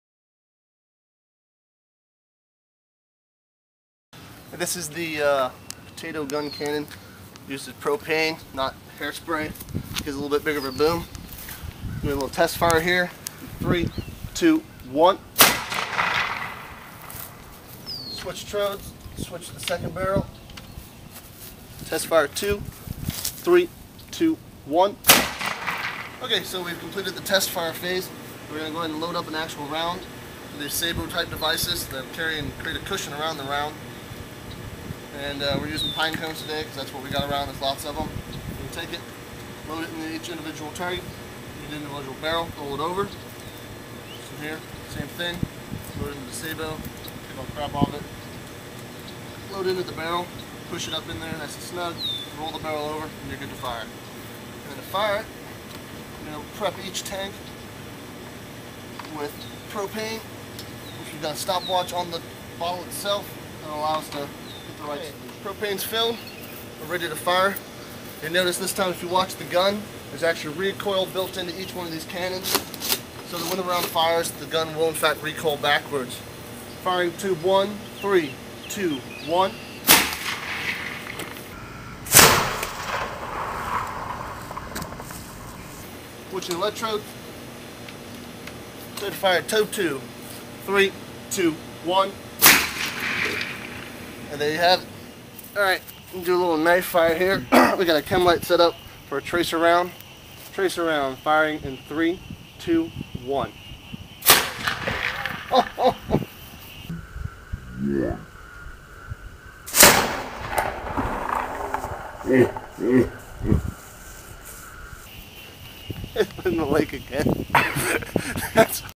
Duel-Barrel Potato Gun w/Recoil
This is a project a buddy and I built in High school. I had to get the principle's permission and an escort by the school cop to bring it on campus. The white things landing in the yard are the sabots not the rounds which you'll see go way off screen. The piece that comes off the right cannon when shot is a safety blow off shroud. It blows off when the round is too big to prevent it from becoming lodged in the barrel. The angle is adjustable and with the sabots it can shoot anything 2 inches or less in diameter. Feel free to post any other questions in the comments and I'll try to answer them for ya.